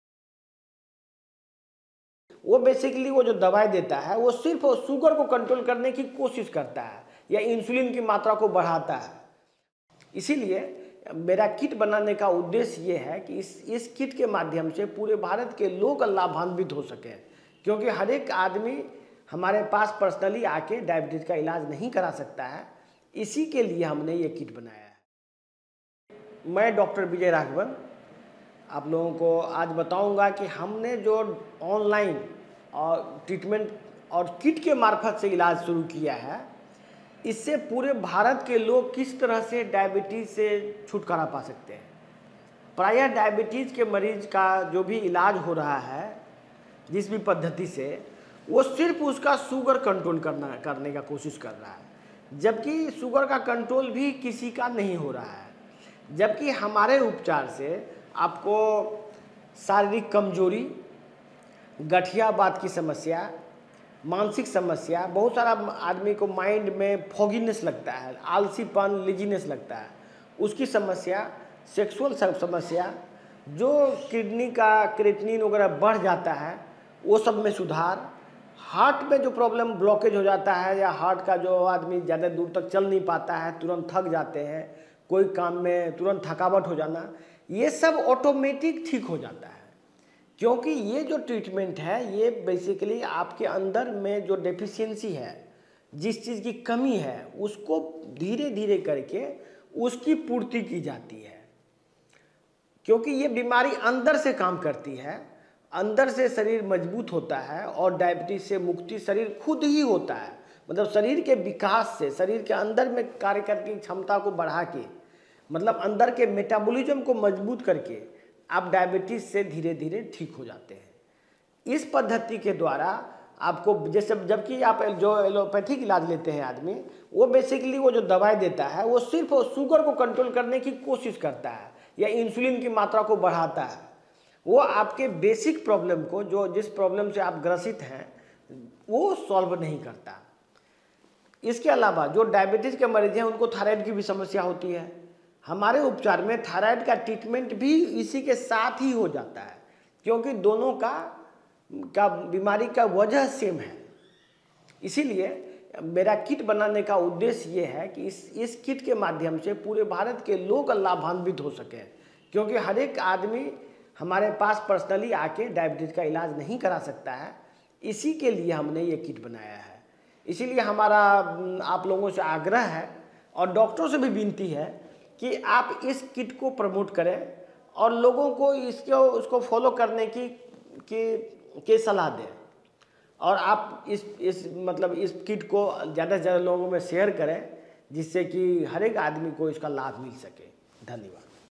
Metabolic Treatment Kit by Dr Vijay Raghavan for diabetes | Best treatment of diabetes
Metabolic Treatment Kit for Diabetes comes with a metabolic diet chart & instructions.

Dr Vijay Raghavan is a well-known researcher of metabolic diseases. He runs Anupama Hospital in Purnea City.

Dr Vijay Raghavan provides metabolic treatment for diabetes, high blood pressure, autoimmune diseases, thyroid, cancer, arthritis, etc.